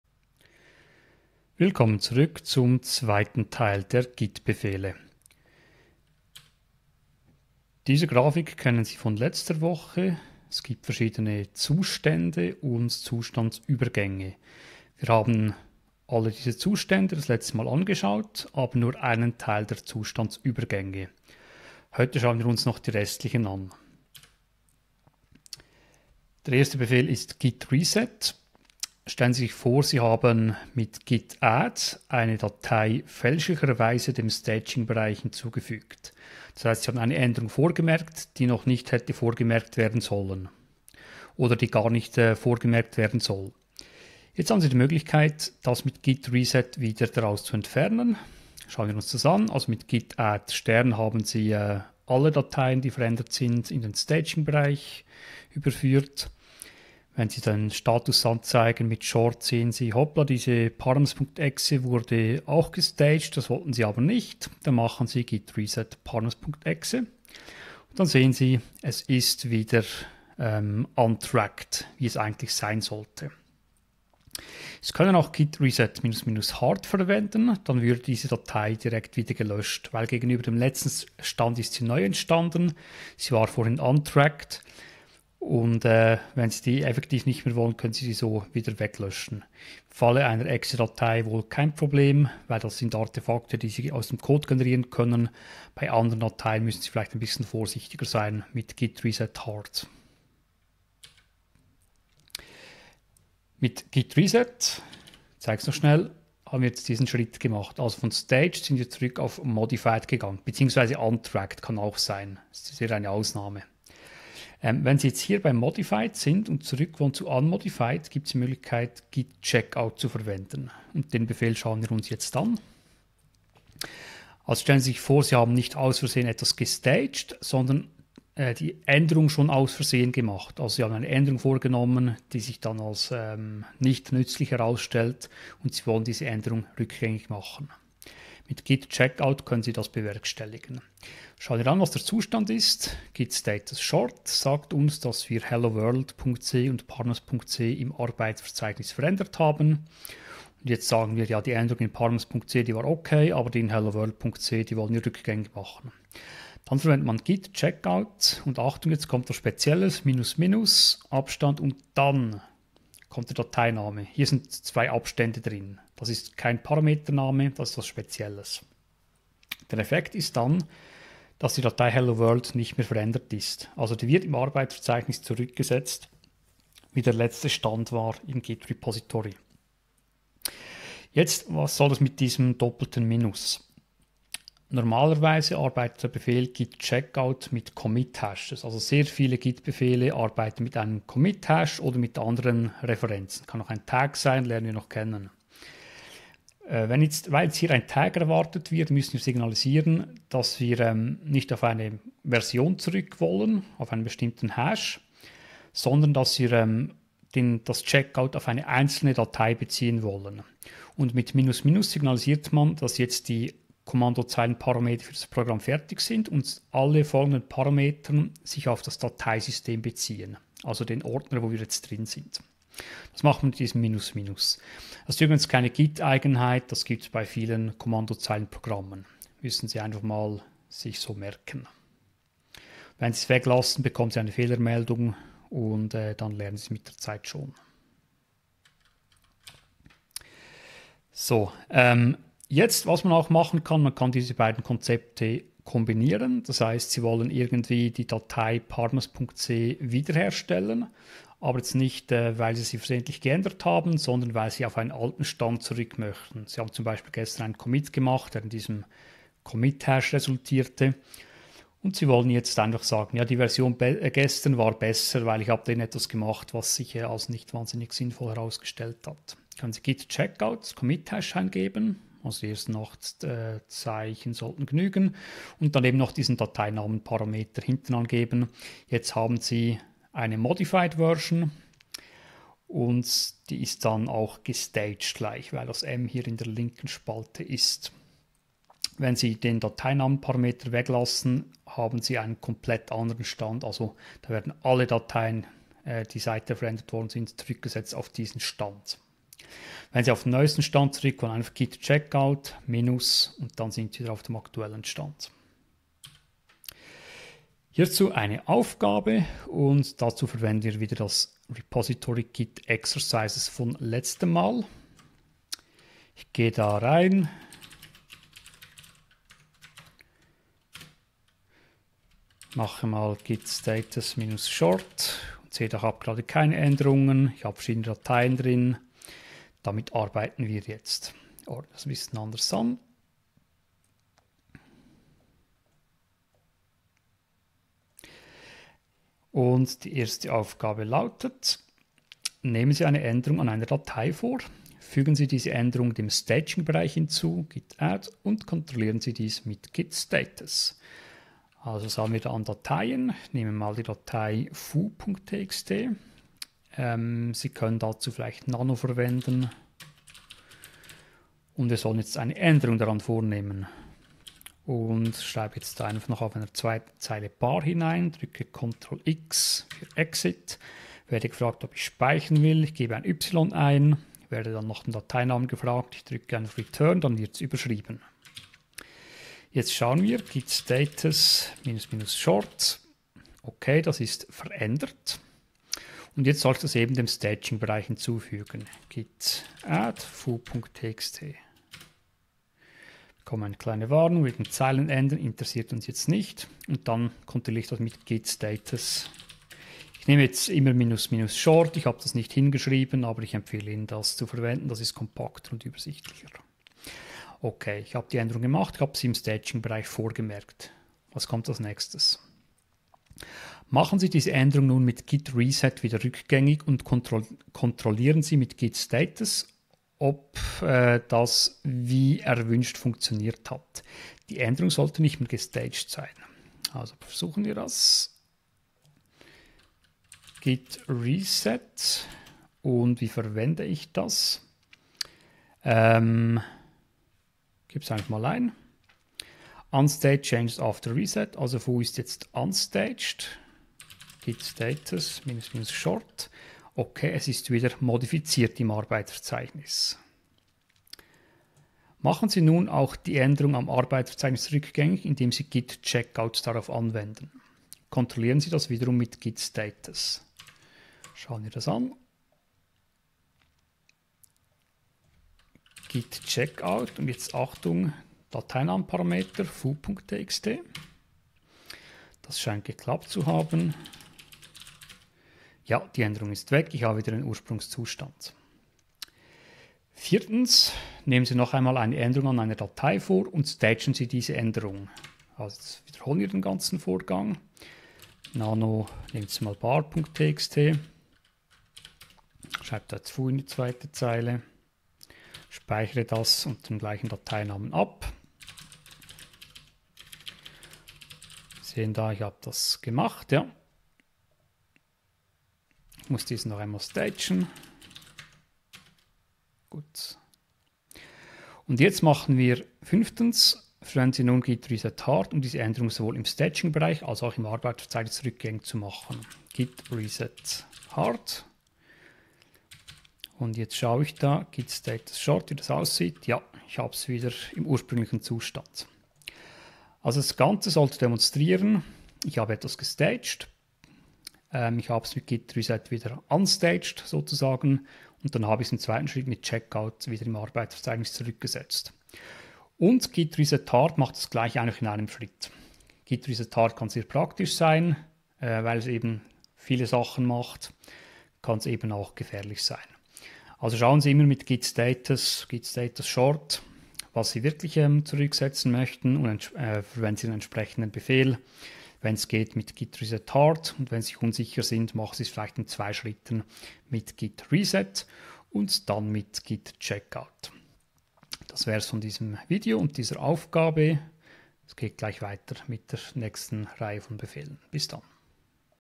9: git checkout und git reset
Mit `git checkout` und `git reset` können Änderungen im Arbeitsverzeichnis bzw. im Staging-Bereich rückgängig gemacht werden.

0:00 Wiederholung: Zustände und Zustandsübergänge
0:24 Theorie: `git reset` (Staging-Bereich zurücksetzen)
1:14 "Hard Reset": Hilfreich, aber mit Vorsicht!
2:04 Theorie: `git checkout` (Arbeitsverzeichnis zurücksetzen)
4:00 `git checkout` auf alten Stand
5:07 Aufgabe 9
8:49 Rückblick